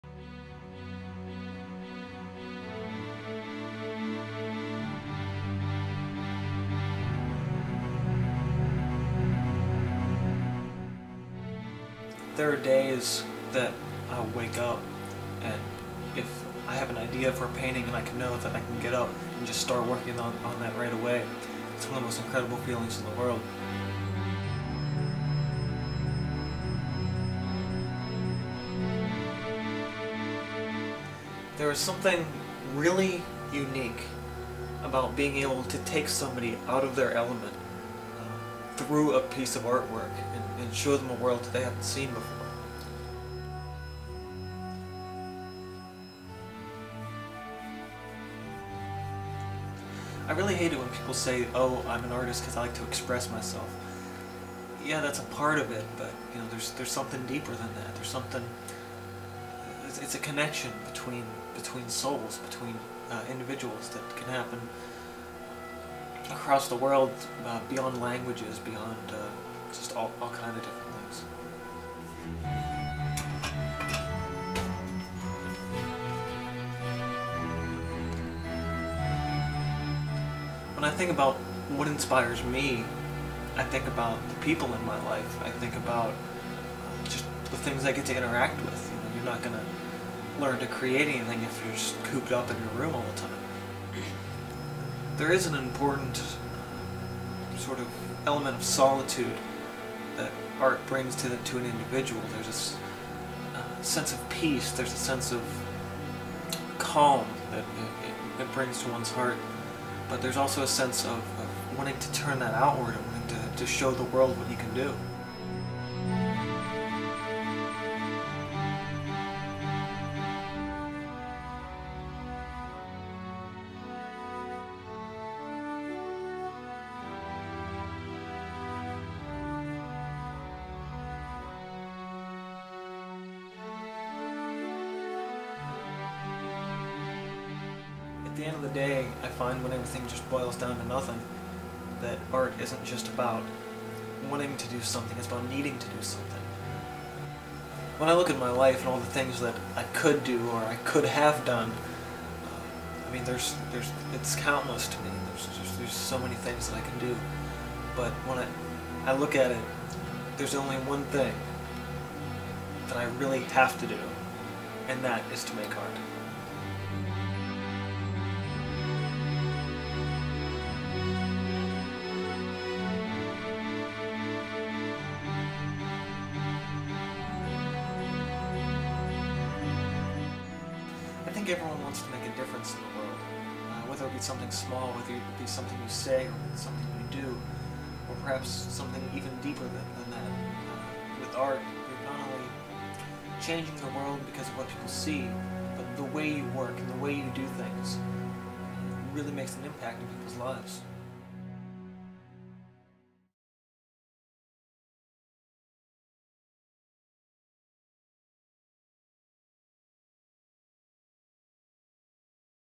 Behind the Artist: Part 1
Part one of what will hopefully be a glimpse in to the man behind the channel. Thanks to Wynn and Hill Smith for the soundtrack (Composers from my Mushi-Shi Fan Film) find more of their music